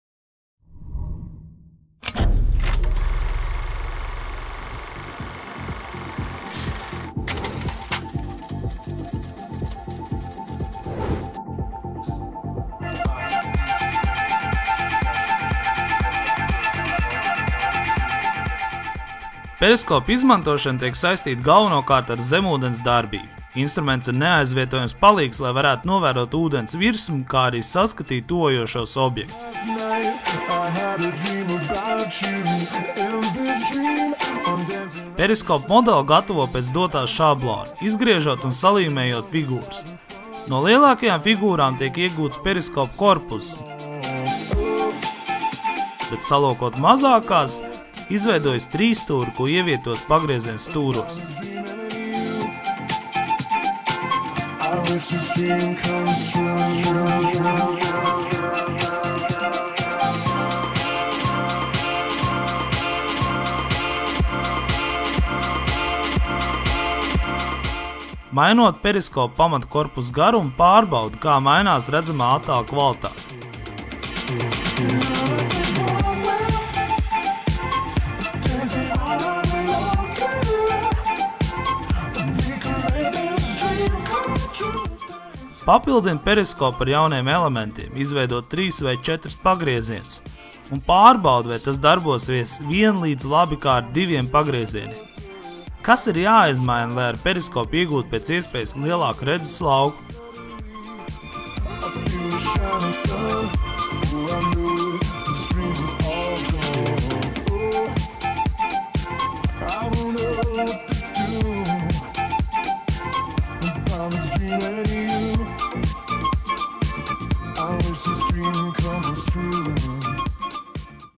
Periskops
Tas snuķītis, kas atrodas uz zemūdenēm un pa kuru tās redz ūdens virspusi, kamēr pašas ir zem ūdens, tiek saukts par periskopu. Ko līdzīgu izveidojām arī mēs.